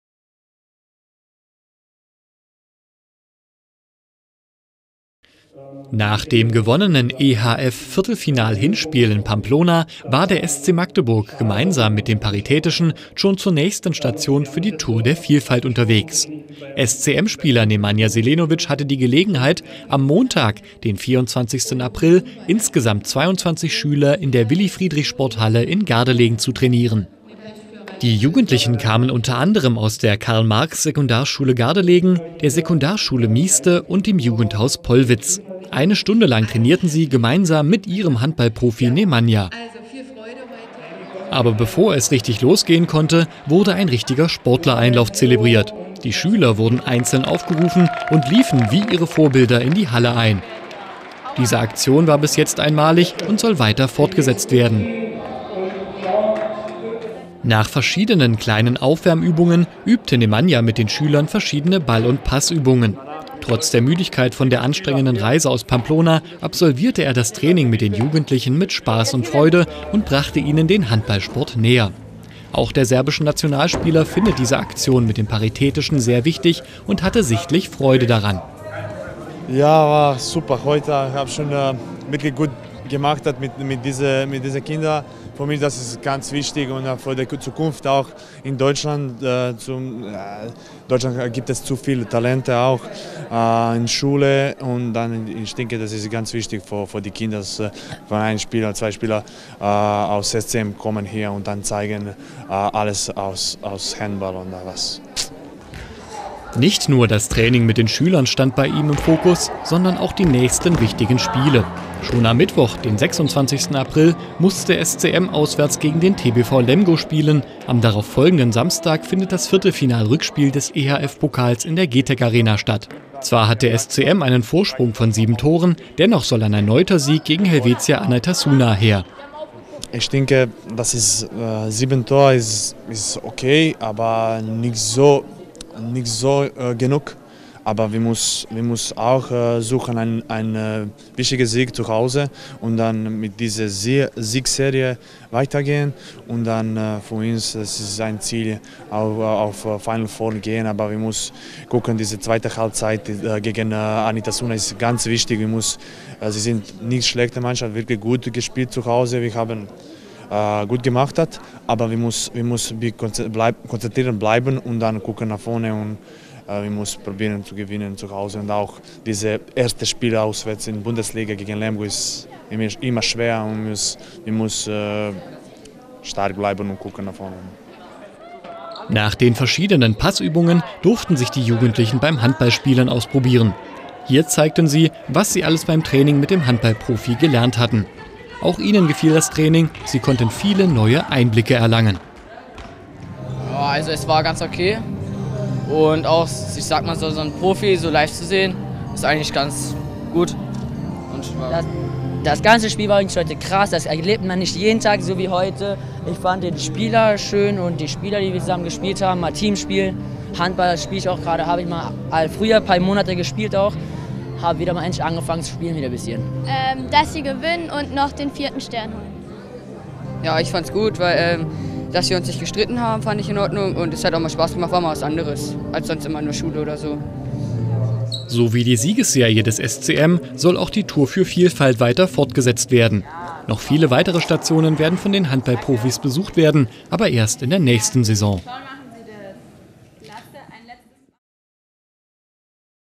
24.04.2017 Nemanja Zelenovic mit der Tour für Vielfalt in Gardelegen
Die vierte Station der Tour für Vielfalt führte nach Gardelegen. Gemeinsam mit SCM-Profi Nemanja Zelenovic trainierten Schüler der 5-10 Klasse eine Stunde Handball. Wir waren mit der Kamera dabei.